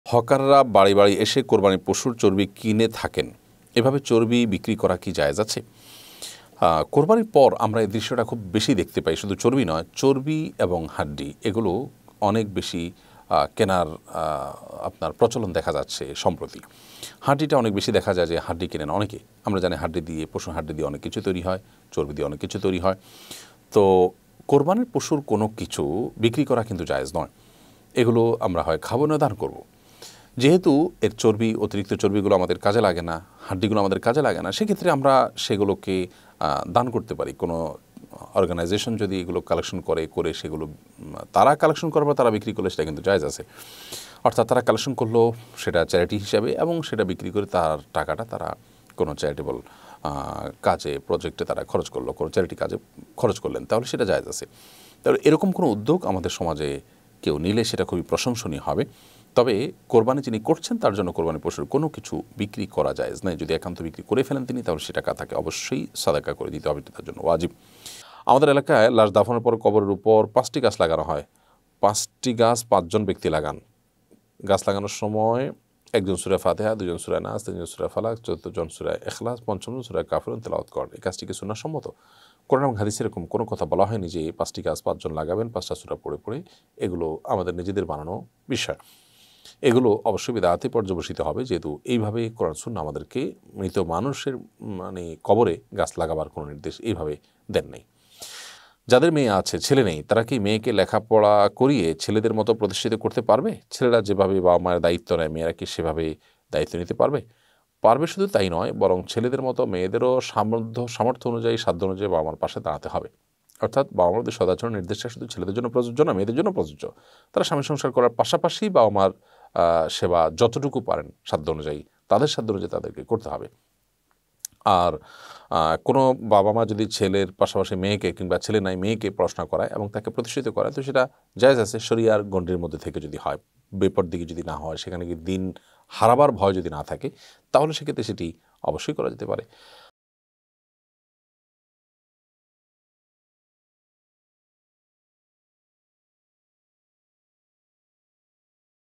হকাররা বাড়ি বাড়ি এসে কুরবানীর পশুর চর্বি কিনে থাকে, চর্বি বিক্রি করা কি জায়েজ?
হকাররা বাড়ি বাড়ি এসে কুরবানীর পশুর চর্বি কিনে থাকে, চর্বি বিক্রি করা কি জায়েজ? new waz II waj II Sheikh ahmadullah